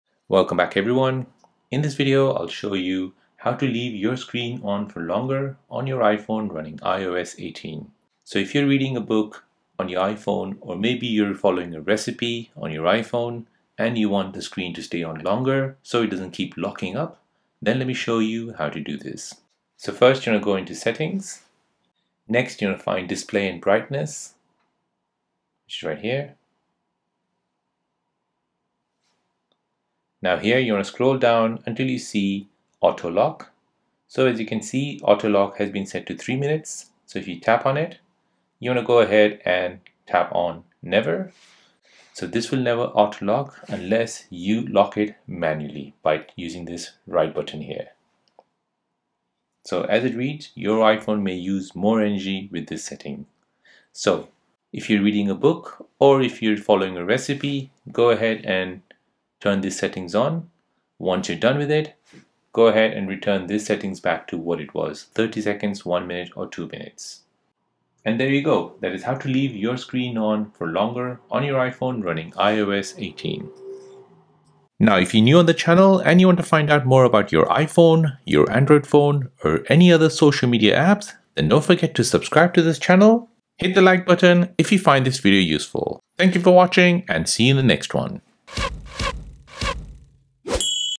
How to Leave iPhone Screen on For Longer | Read a Book Or Recipe | iOS 18 Tutorial 2024
00:00 Intro
00:10 Like or no Like
00:20 How to Leave iPhone Screen on For Longer